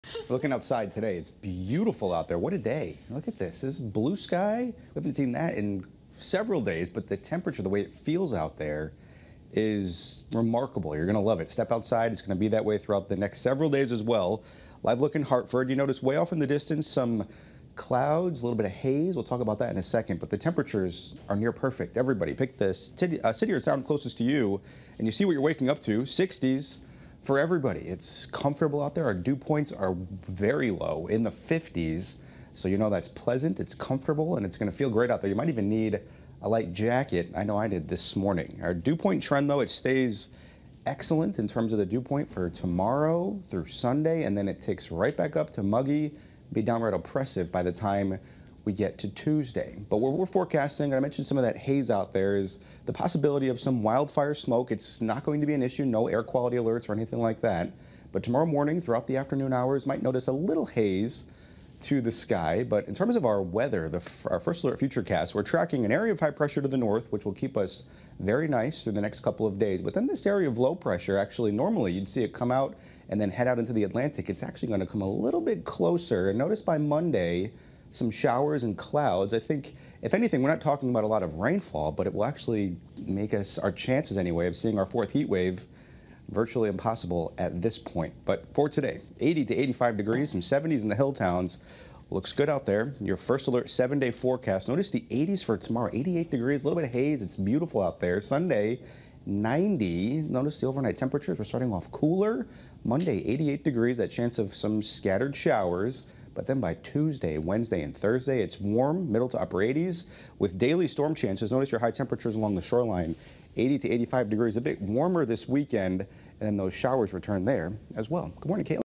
FORECAST: A break from unsettled, humid weather will last a few days
Meteorologist Scott Gagliardi said we're in a break from the unsettled/humid weather. Here's his Friday 7:20 a.m. forecast.